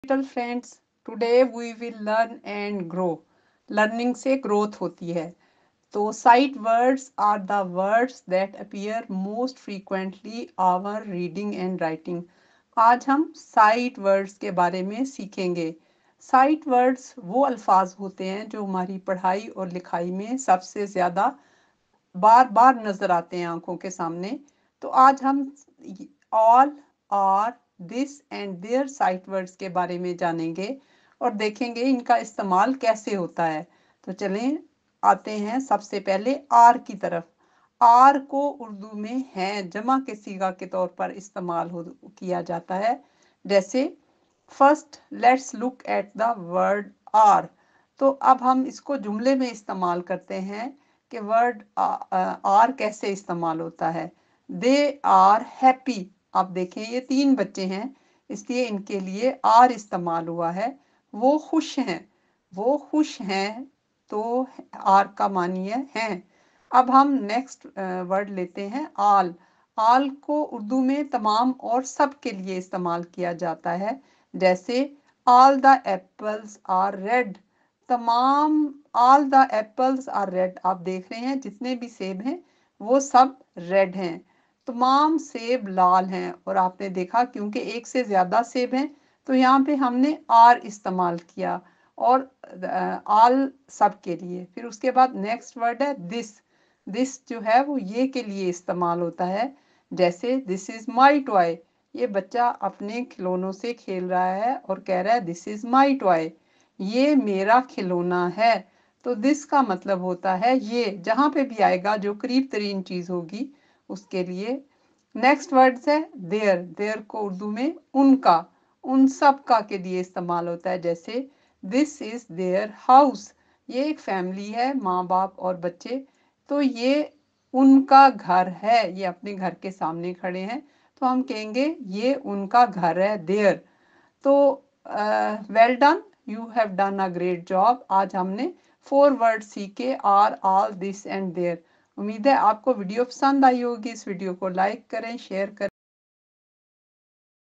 Essential Sight Words for Early Learners
"🌟 Welcome to our exciting lesson on Sight Words for Class 1 Kids with Urdu Translation! In this video, we will help young learners recognize and understand the most important sight words. These Sight Words for Class 1 Kids with Urdu Translation include are, all, this, and their.

Sight words appear frequently in reading and writing, so it’s important for students to learn them well. Through this video, we focus on Sight Words for Class 1 Kids with Urdu Translation to make learning easy and fun.

Kids will get to see examples and colorful visuals to help them understand these words. By the end, your little ones will be confident in using these Sight Words for Class 1 Kids with Urdu Translation in their daily sentences!

Let’s make learning fun together with Sight Words for Class 1 Kids with Urdu Translation.